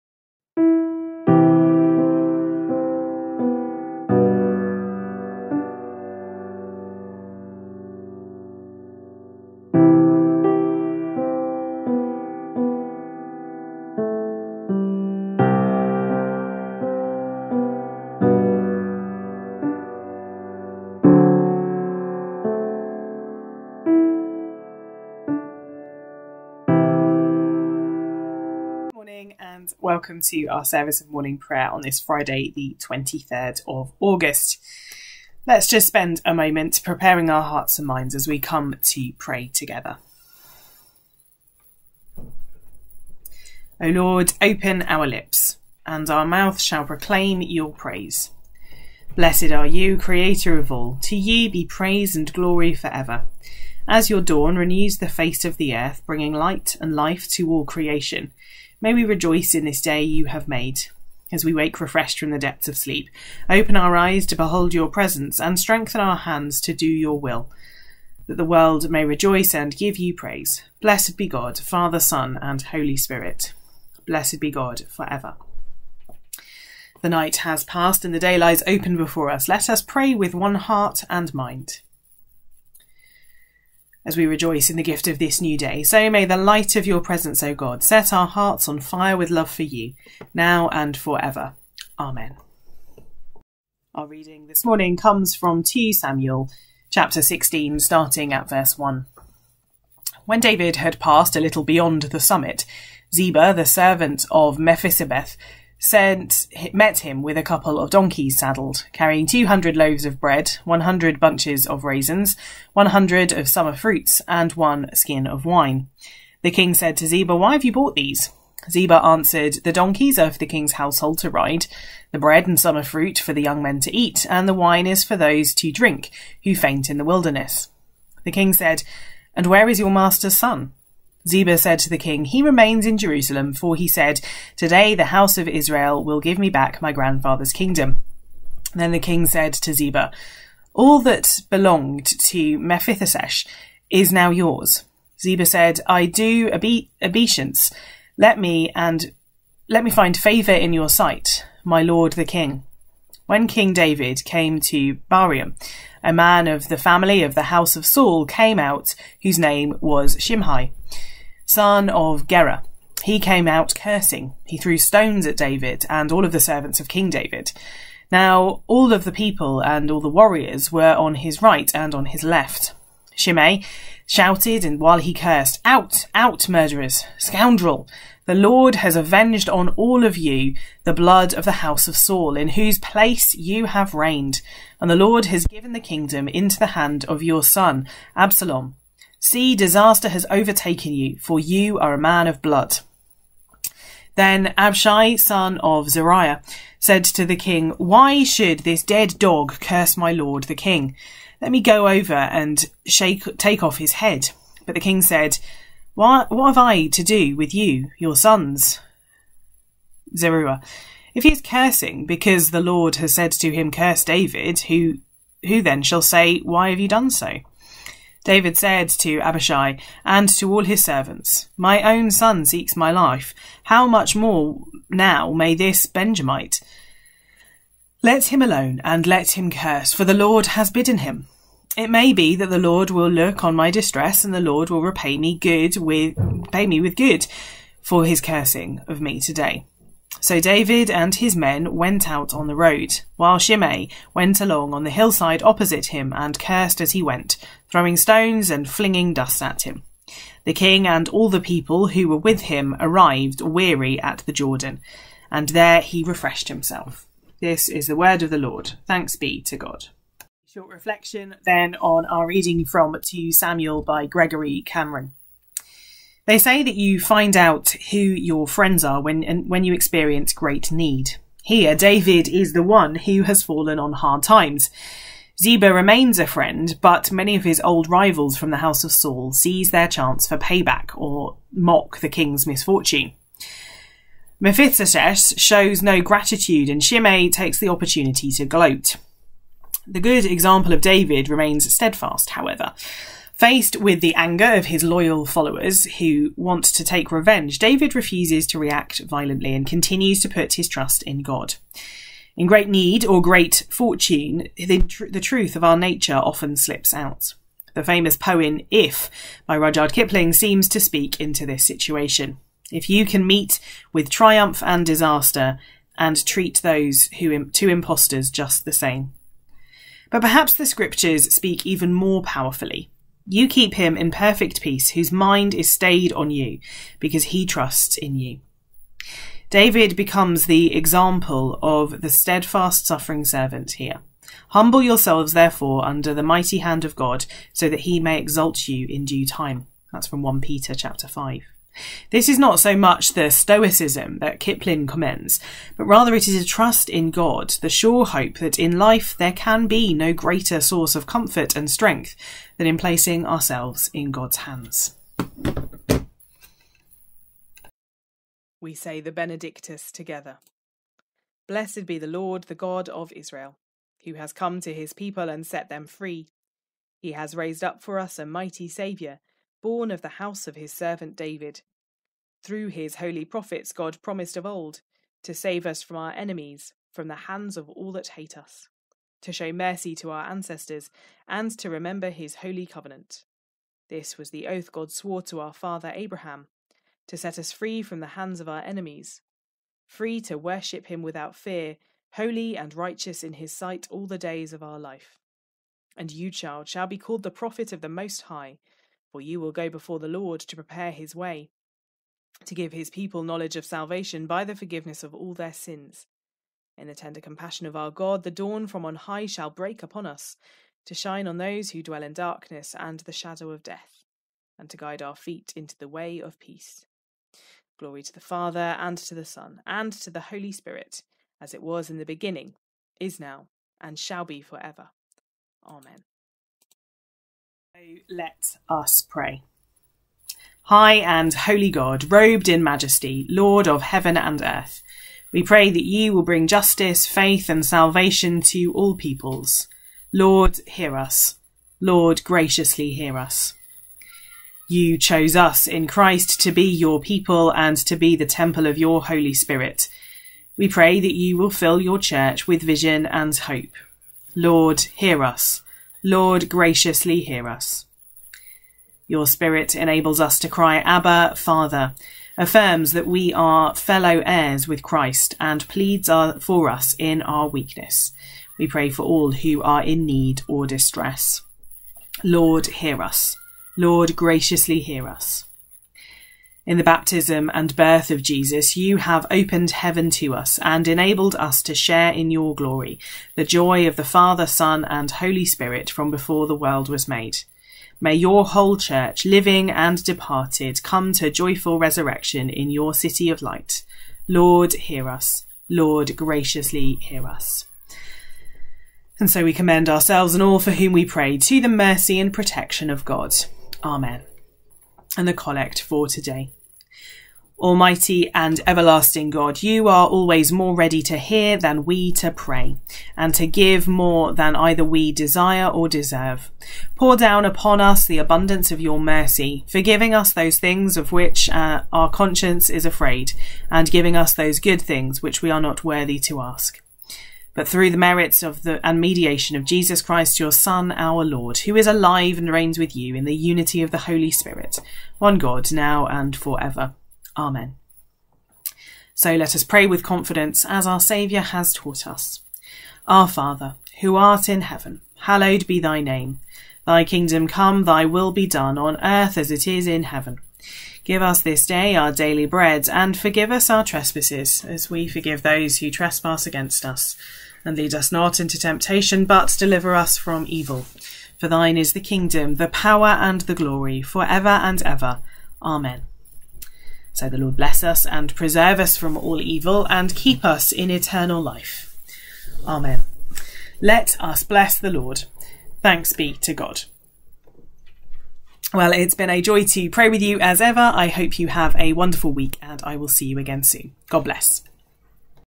Morning Prayer Friday 23rd August
Join Revd Lex for this service of Morning Prayer.